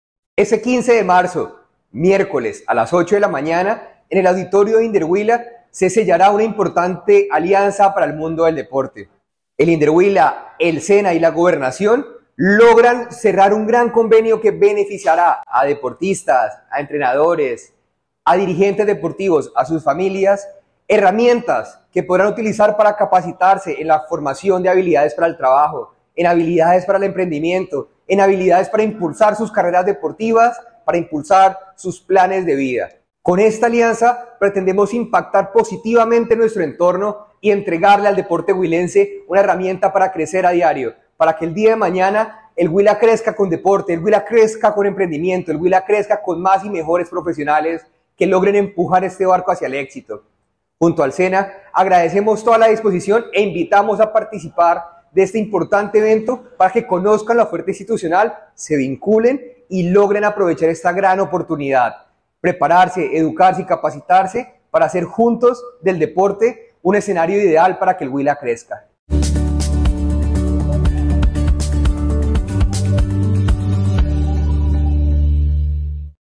SOCIALIZACIÓN DE LA OFERTA DEL SENA HUILA AL SECTOR DEPORTIVO.
Deportistas, entrenadores, dirigentes y sus familias, invitados a la socialización de la oferta académica del SENA Huila, para adelantar sus carreras técnicas, este miércoles 15 de marzo a partir de las 8:00AM, en el salón Centenario del Inderhuila.